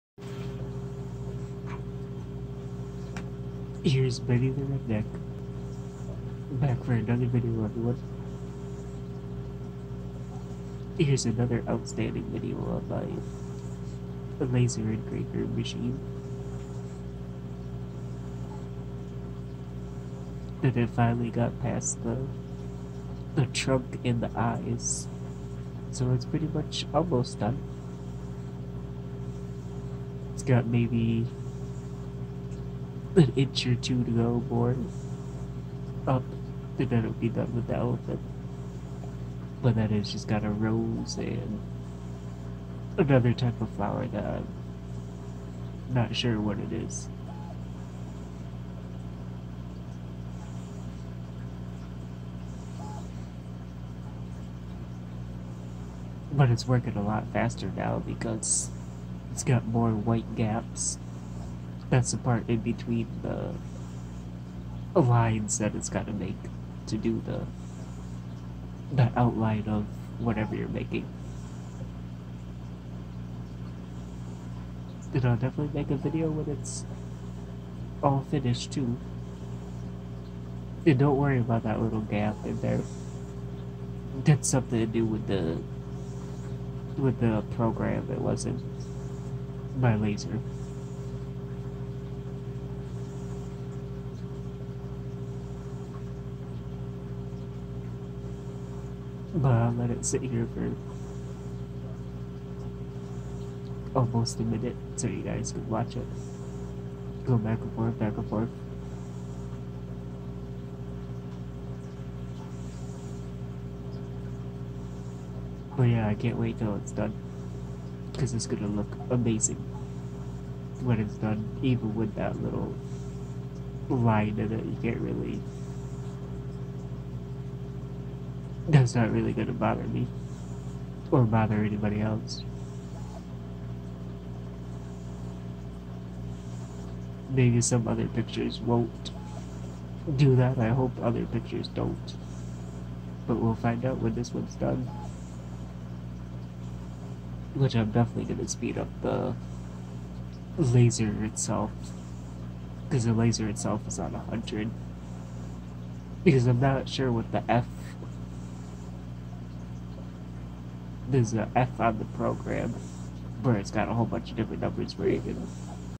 ORTUR laser master 2 S2 engraver episode 4 finally got past the trunk and the eyes
sorry the video got cut short it's because I got to get more storage so I hope you guys still enjoy the video